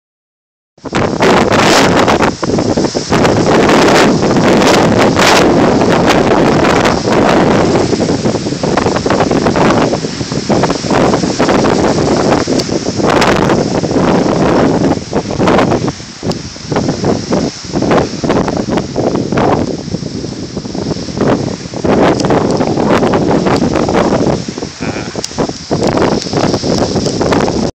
Monsoon Dusts Storm Rainbow
Monsoon dust storm in mesa, az. Theres a rainbow at the beginning of the storm.